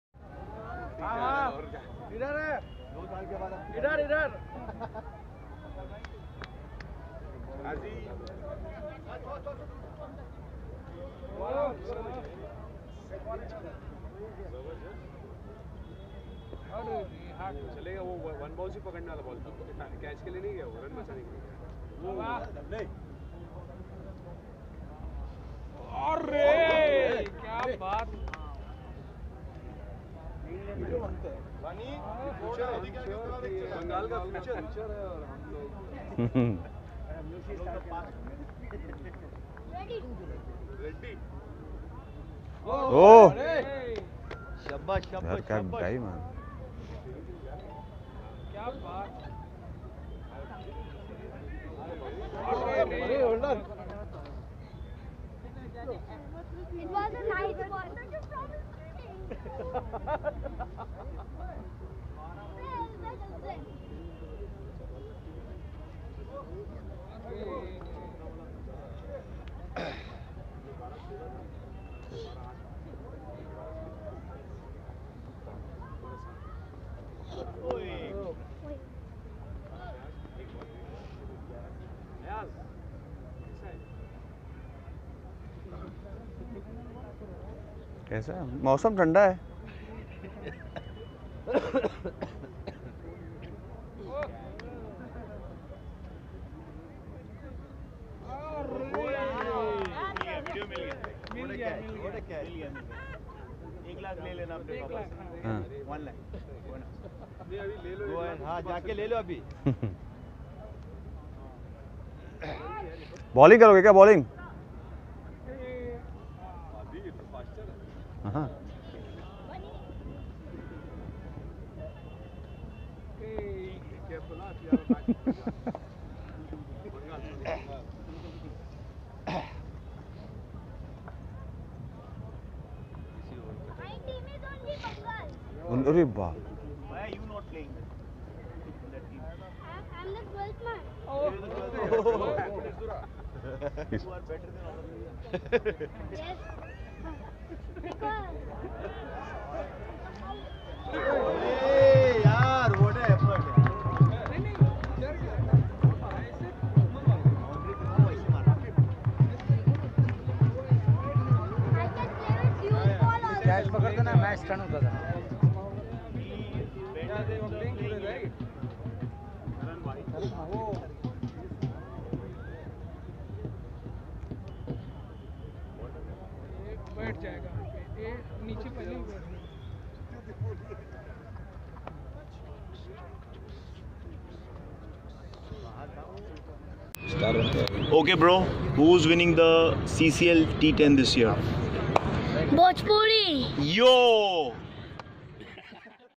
CCL10.BANGAL TIGER VS KARNATK BULDOSER MATCH ME बच्चे का कमाल
CCL 10 bangal vs karnatk match के दौरान बच्चे का प्रैक्टिस देखिए